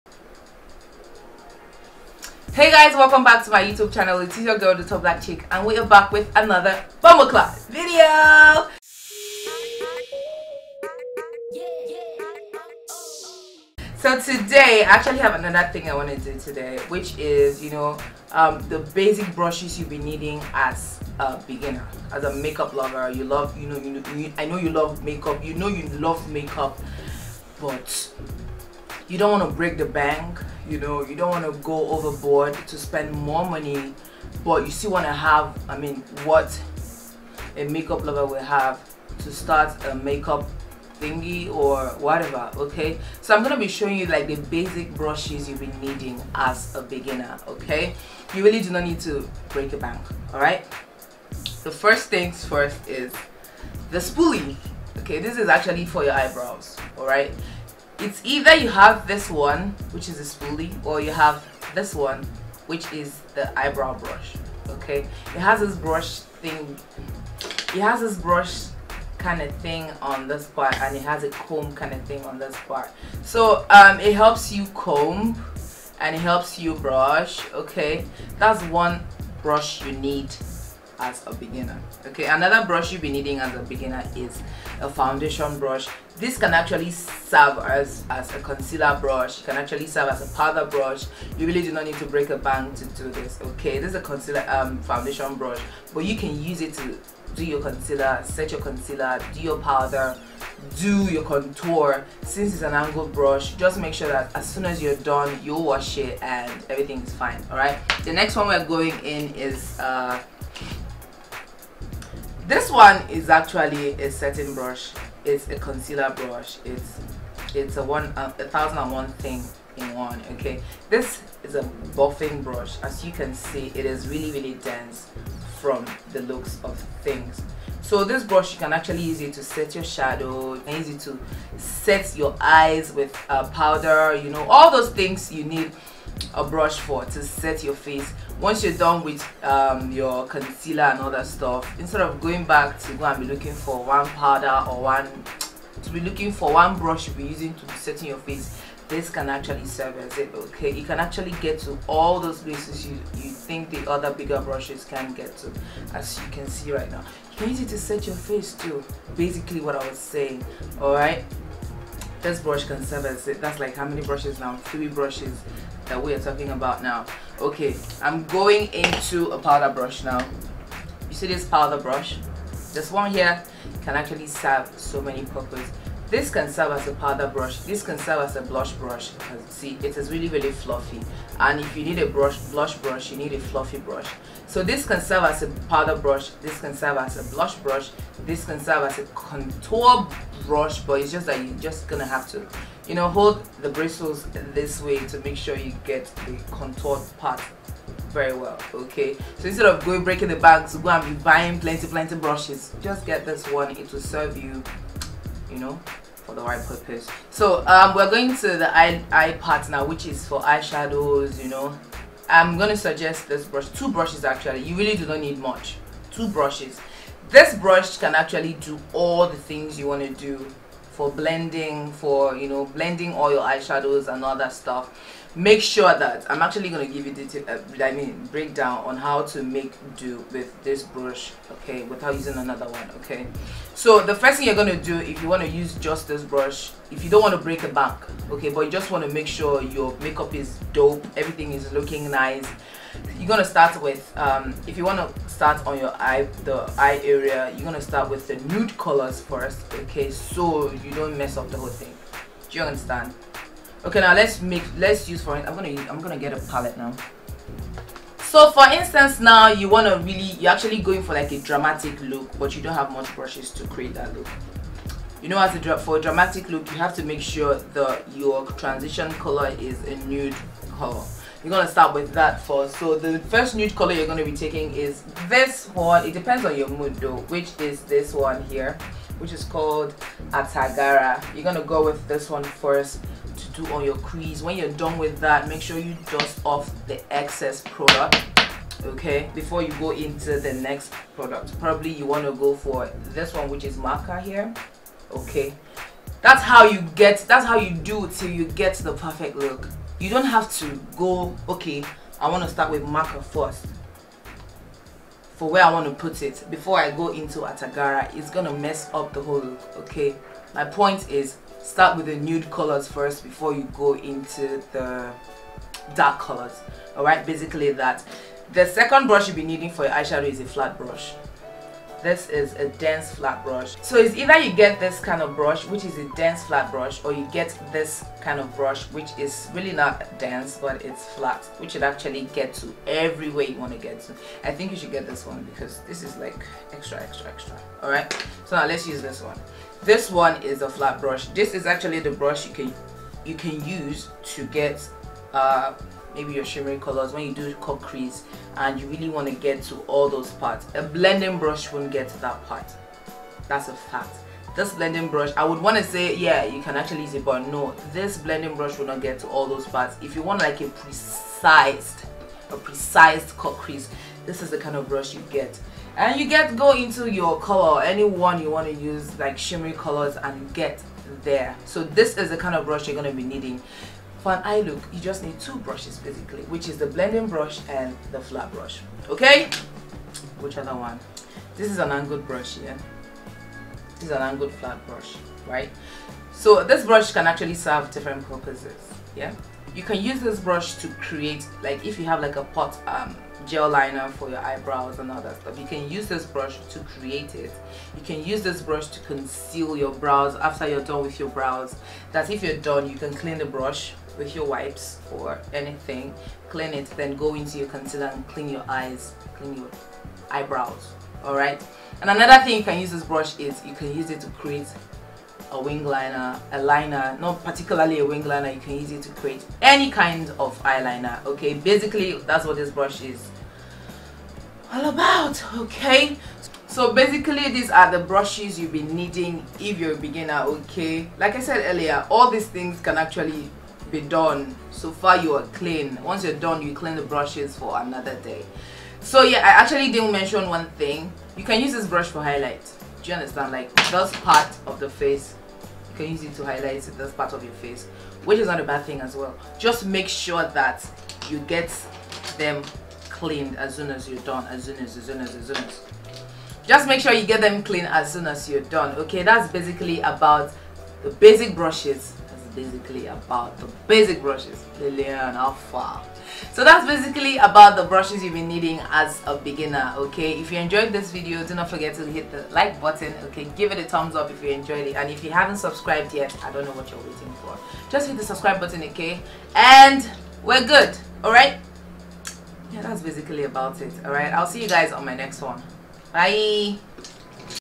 BASIC MAKEUP BRUSHES FOR BEGINNERS AND THEIR USES// THETALLBLACKCHIC
Hey Dolls!
Its a new video Up!
In this video i did the Basic Brushes for beginners, and it’s really important you know the basic brushes as a beginner and their uses.